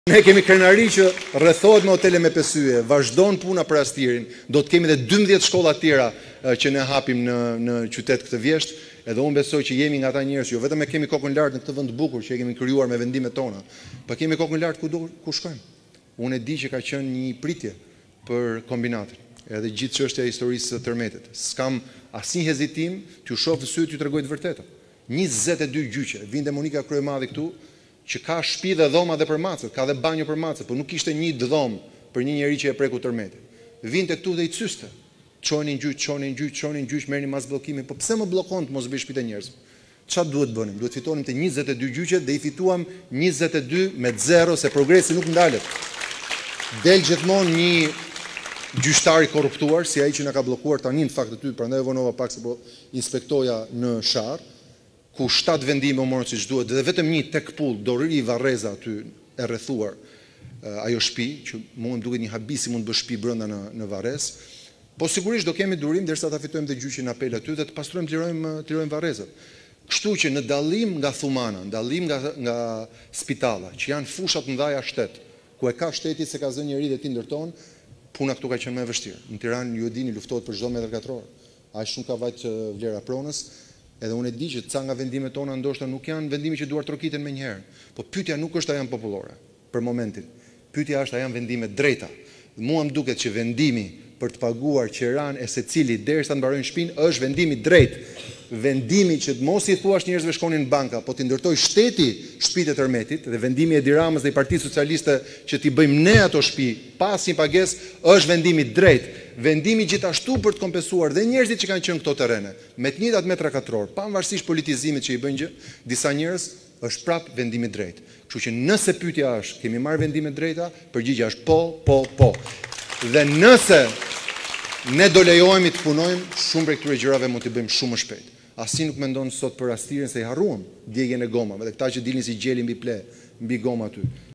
Veliaj ironizon Kryemadhin: Ajo ka dhoma dhe banjo për macet, por jo për familjet e prekura tërmeti
reportTV lajmeshqip lajme Kryetari i Bashkisë së Tiranës, Erion Veliaj ka ironizuar Kryetaren e LSI, Monika Kryemadhi, në lidhje me akuzat e saj mbi procesin e rindërtimit. Nga Kombinati, ku zhvilloi një takim me strukturat e PS, Veliaj tha se shqetësimi i vetëm i Kryemadhit është interesi i saj politik.

“Kombinati ka pritur gjatë për çështjen e tërmetit dhe unë dua t’ju shoh në sy e t’ju them të vërtetën: Kemi bërë 22 gjyqe. Këtu vinte Monika Kryemadhi - që ka shtëpi e dhoma edhe për macet, ka edhe banjo për macet, por nuk ka një dhomë për një familje të prekur nga tërmeti - dhe i cyste njerëzit, u thoshte çojeni në gjyq, merrni masa bllokimi. Po pse më bllokon të mos bëj shtëpitë e njerëzve? Çfarë duhet të bënim? Duhet të fitonim 22 gjyqet dhe i fituam 22 me 0, se progresi nuk ndalet”, u shpreh ai.

Veliaj shprehu bindjen se Bashkia e Tiranës do të fitojë edhe gjyqin për të zhbllokuar punën për heqjen e ndërtimeve pa leje te varrezat e Sharrës.